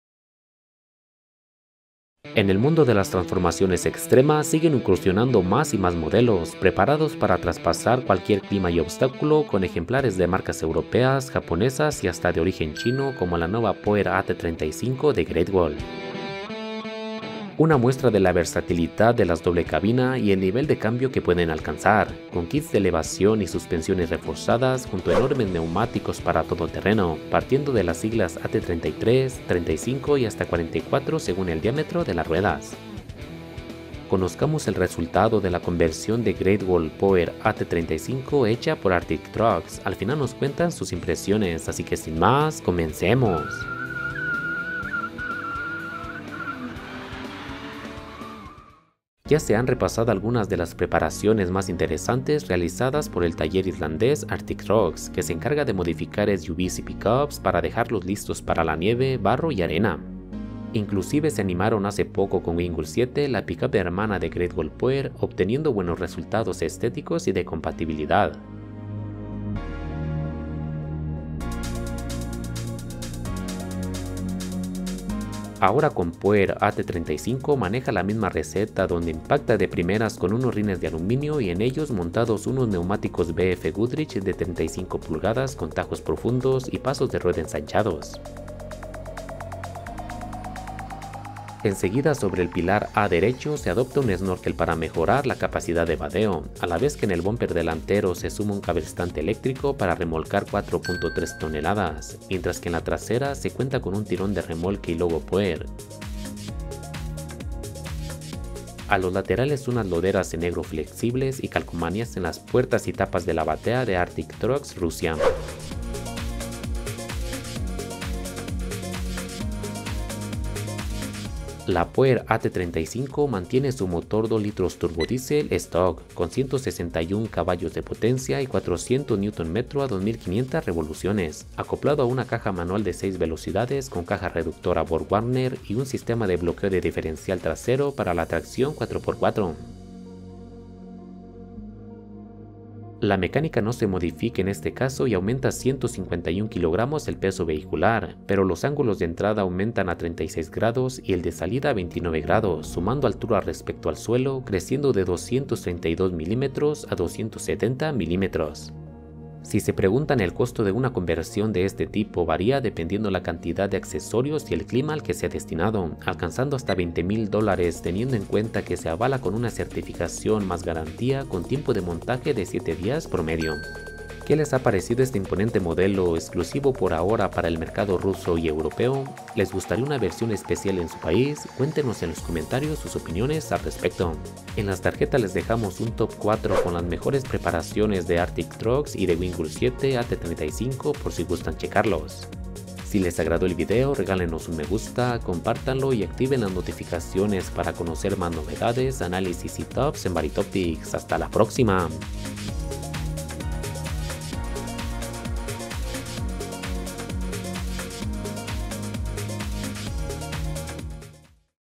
Great Wall POER AT35🐉 by Artic Trucks ¡BESTIA China! | Vary Topics
Conoce un Pick Up China by Artic Trucks, GWM POER + Ruda y Capaz para 4x4 como Wingle 7 AT35 ¿Mejores que Navara, L200, Wingle 7 y Hilux? Cuéntanos que te parecen.